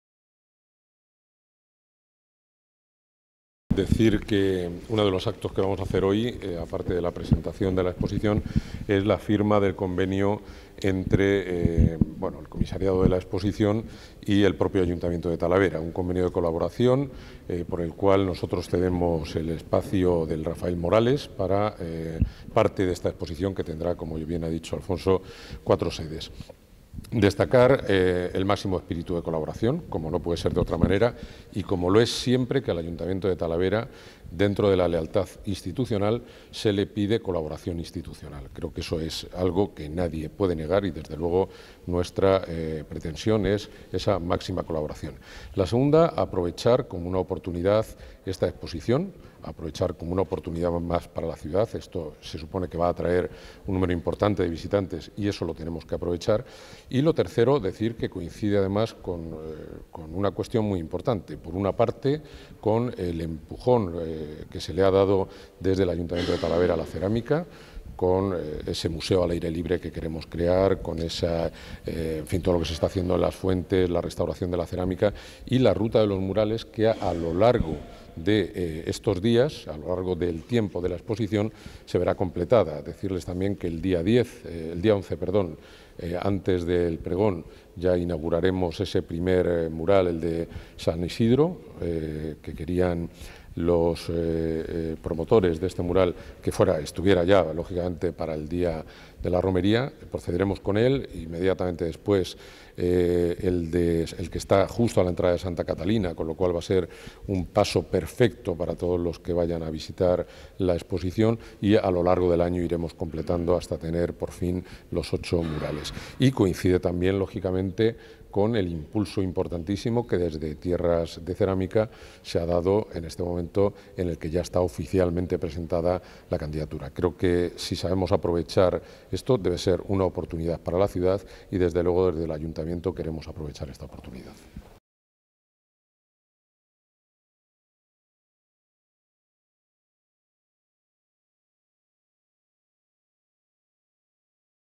aTempora Talavera Jaime Ramos, Alcalde de Talavera de la Reina
aTempora Talavera, 6.000 años de Cerámica en Castilla La Mancha
Presentación de la exposición, que tendrá lugar de septiembre de 2018 a enero de 2019 y, firma del convenio de colaboración entre el Ayuntamiento de Talavera de la Reina y la Junta de Castilla La Mancha.
Jaime Ramos, Alcalde de Talavera.